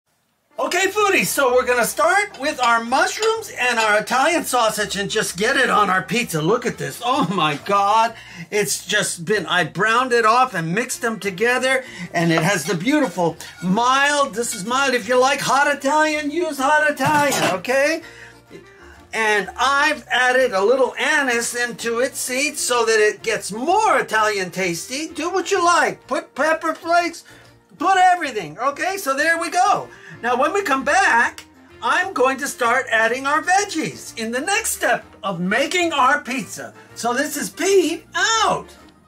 Pizza Pete Is Global & Building A Pizza 5#shortviral
Pizza Pete is global building pizza Pizza's a global phenomenon, but its roots are ancient.Flatbreads with toppings go back to Egyptians and Romans. Fast forward to Naples, Italy, 18th century. Poor folks needed cheap eats, and pizza - portable, filling, with simple ingredients - became a hit.